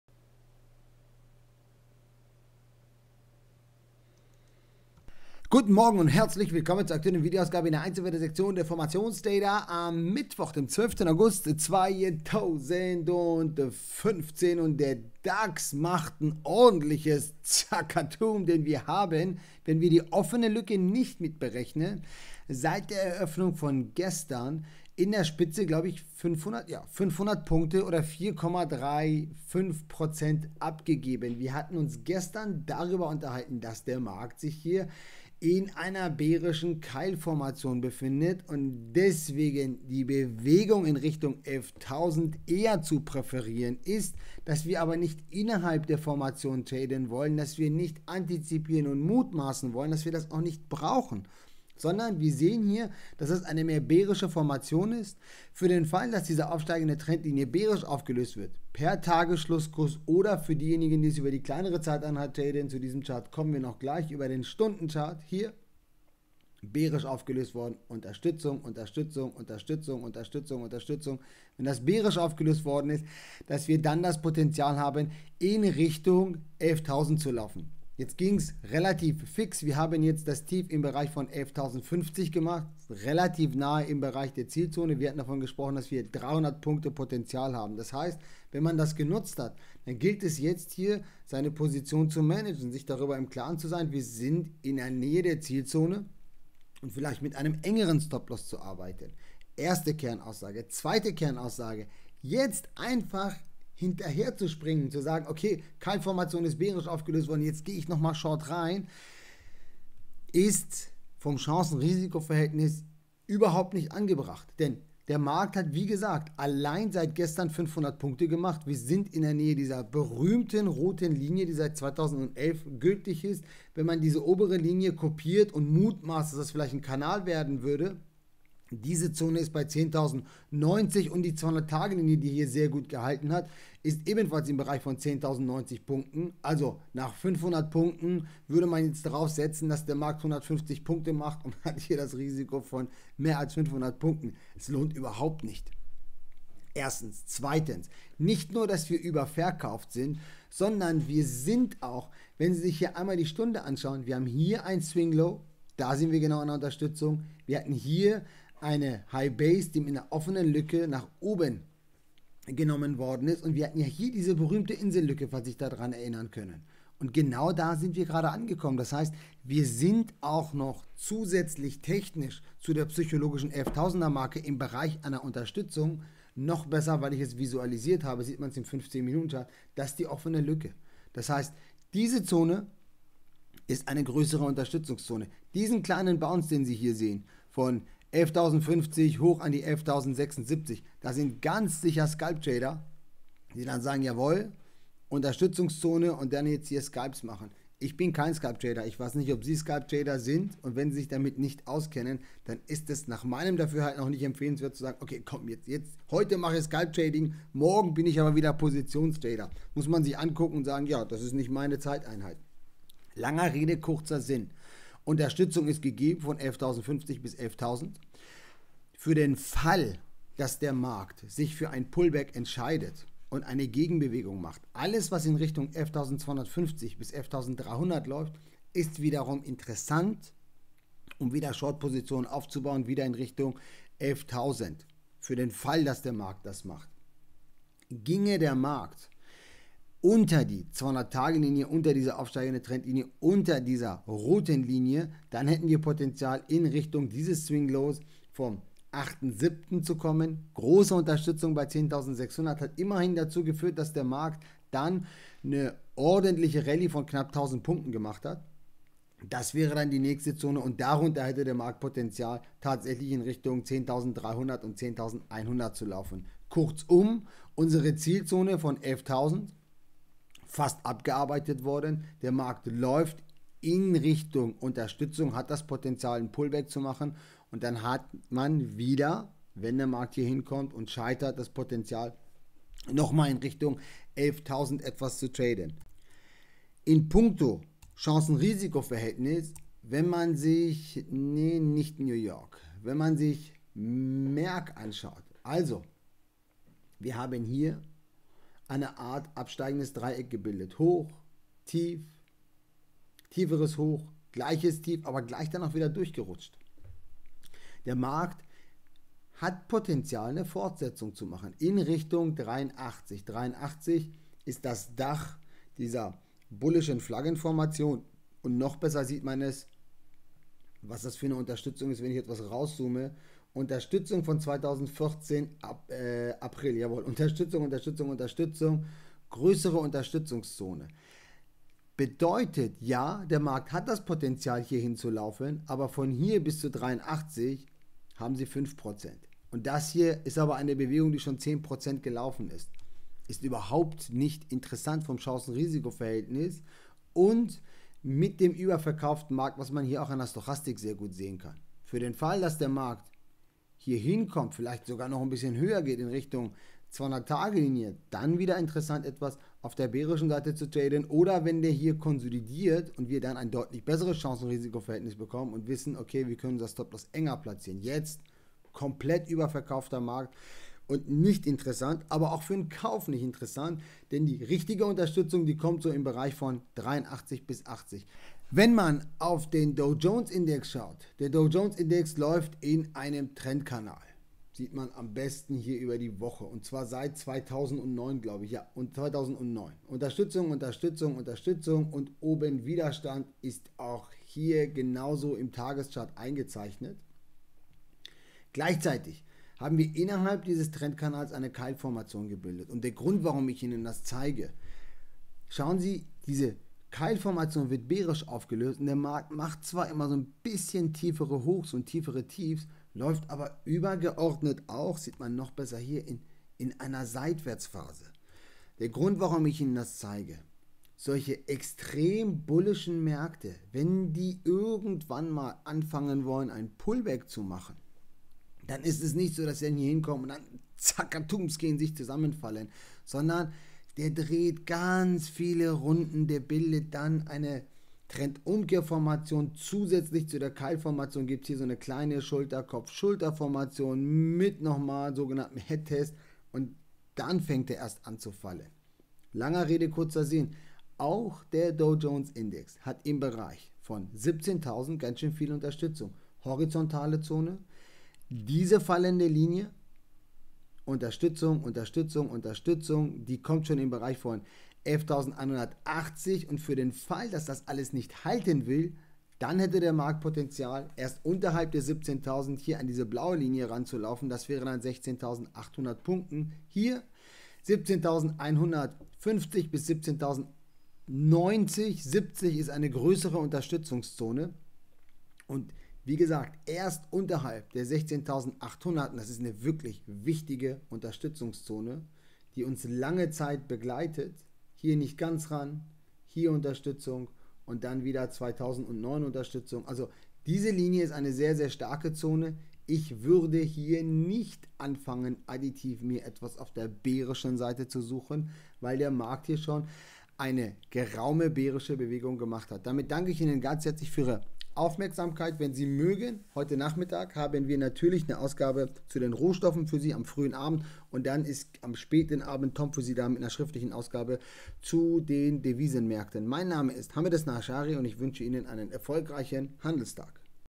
Dax: das hatten wir erwartet
Guten morgen,
an dieser Stelle hatten wir gestern über die größere Wahrscheinlichkeit einer bärischen Bewegung beim Dax aufgrund der bärischen Keilformation gesprochen und dass unterhalb der 11.300 die 11.000 aktiviert worden ist.
Die Zielzone wurde mittlerweile erreicht.

Die aktuelle technische Konstellation aus unserer Sicht  beim Dax, Dow Jones und Merck ist Gegenstand der heutigen Ausgabe.

Hamed Esnaashari